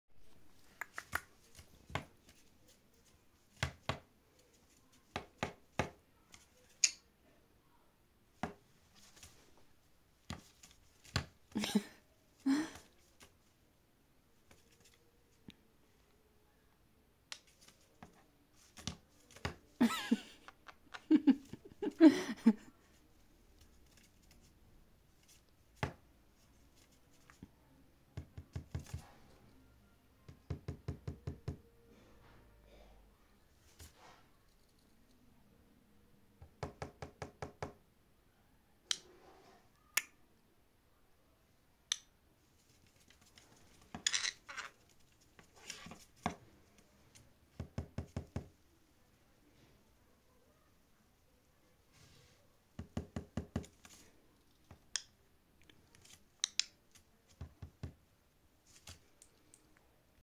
CONURE GETS ANGRY AND JUMP!!!
My conure gets angry to my hand and jump trying to bite me!!😣